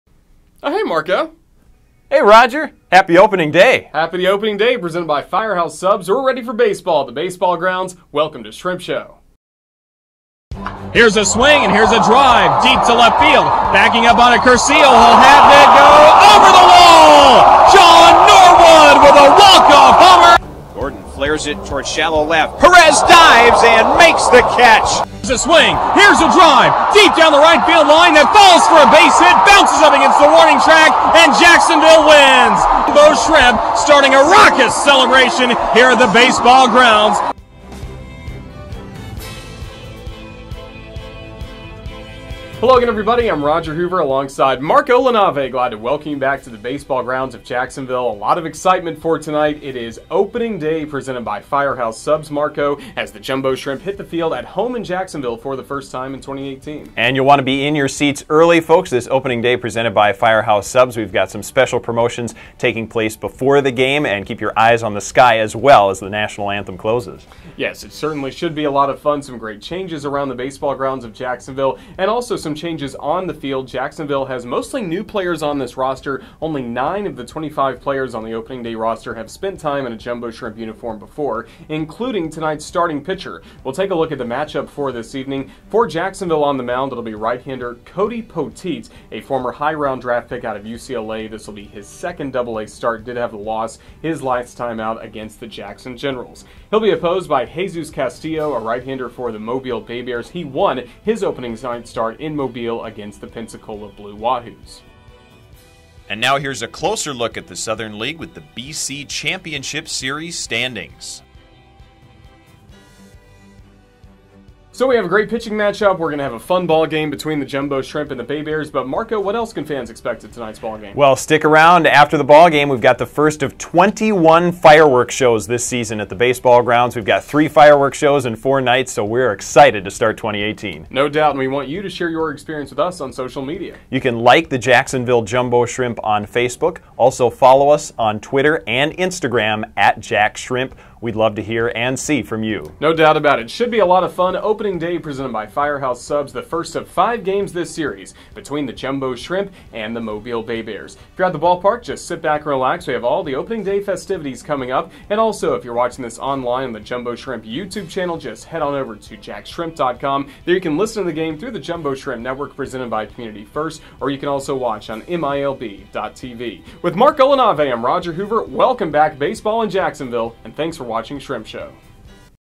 Shrimp Show - April 11, 2018
Join Shrimp broadcasters Roger Hoover & Marco LaNave as they preview at the 2018 home opener at the Baseball Grounds of Jacksonville in Shrimp Show.